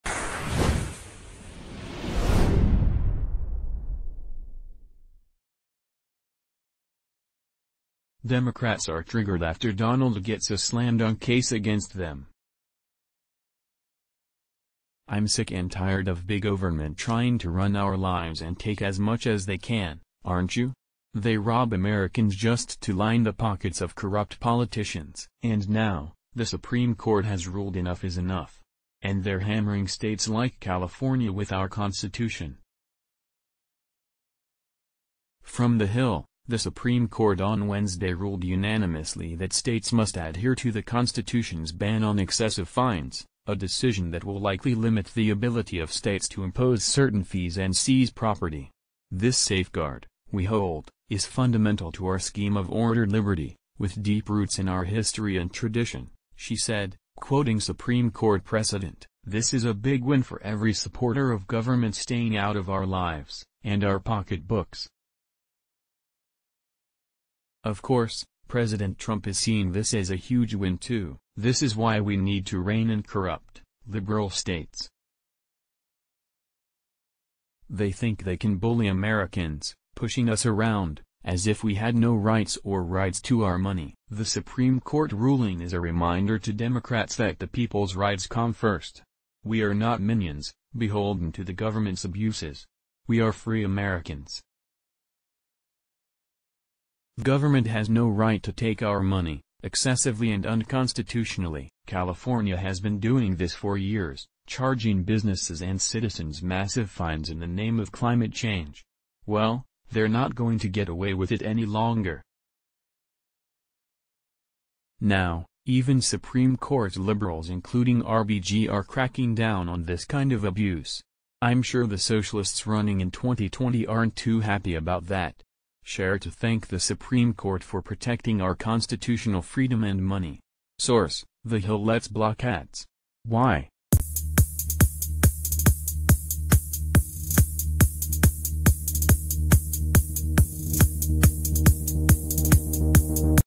In Historic 9-0 Decision, Supreme Court Drops Constitution On California
Democrats are triggered after Donald gets a slam-dunk case against them. I’m sick and tired of big overnment trying to run our lives and take as much as they can—aren’t you? They rob Americans just to line the pockets of corrupt politicians. And now, the Supreme Court has ruled enough is enough. And they’re hammering states like California with our Constitution. From The Hill: The Supreme Court on...